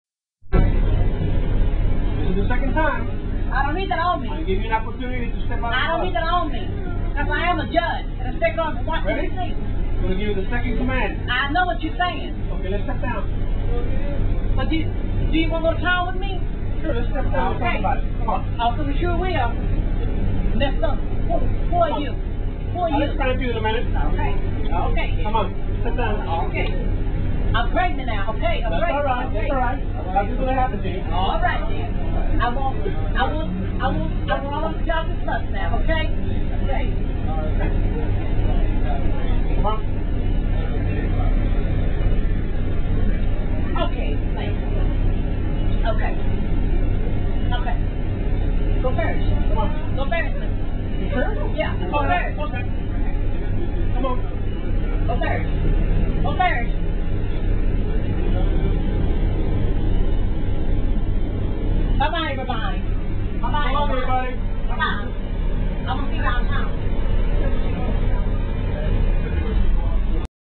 Metro Chaos
I got on the bus to go home from school. And we came to a stop. All of a sudden a Metro Police cop car came infront of the bus and the officer came to his woman and asked her to get off the bus. The woman was just sitting there quitly prior to this. She also had a Holy Bible with her. Poor thing. When she finally got off the bus, she walked in the opposite direction of the officer. LoL